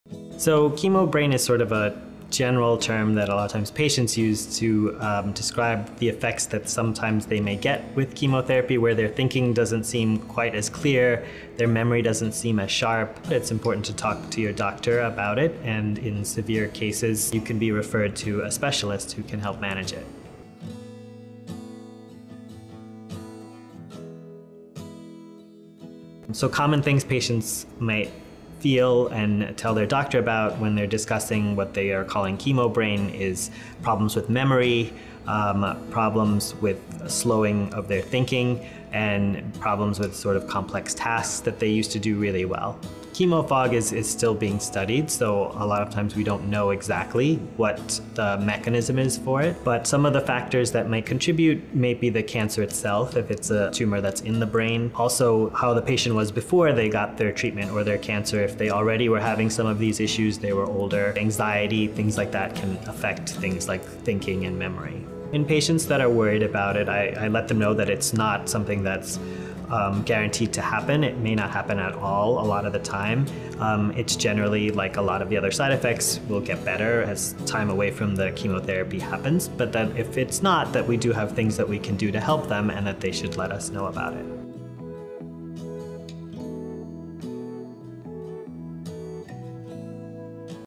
What Is “Chemo Brain” and How Long Does It Last? | GoodRx
Chemotherapy comes with many side effects, such as hair loss and nausea. One side effect that is lesser known is “chemo brain,” which refers to cognitive changes during chemotherapy and other treatments for cancer. In this video, learn what chemo brain is and what causes it, as well as tips to cope with chemo brain during cancer treatment.